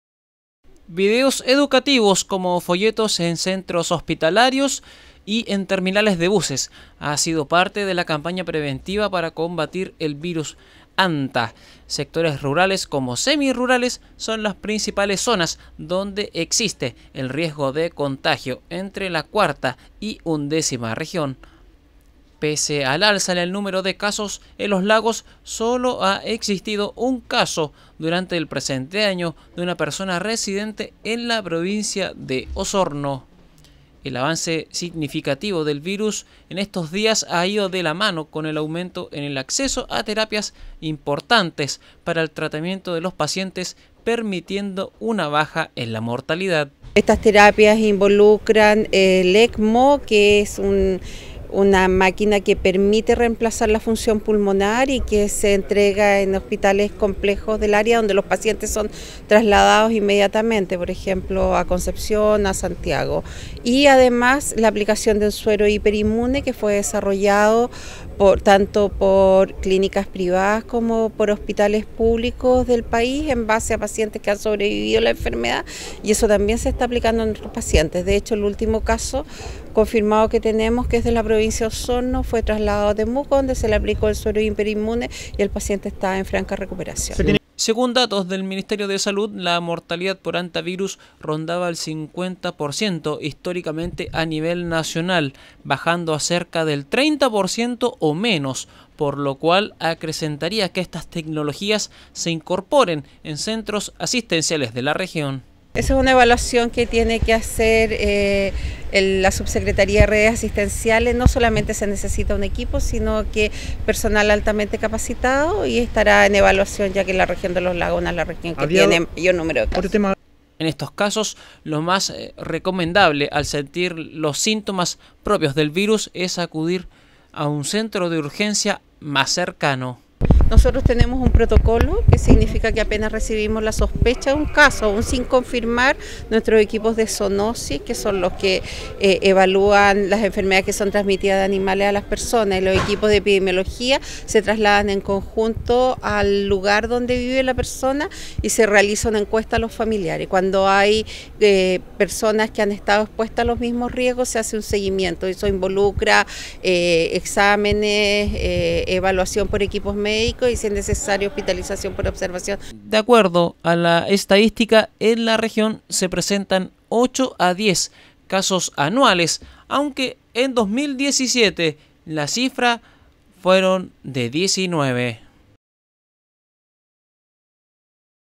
Se mantiene la precaución por casos de Hanta Virus en la región.
Durante este período estival, la Seremi de Salud en Los Lagos ha indicado sólo un caso de Hanta Virus en la región, la cual es una persona de la Provincia de Osorno. La autoridad ha reforzado la educación y prevención debido al alto riesgo de contagio en sectores rurales.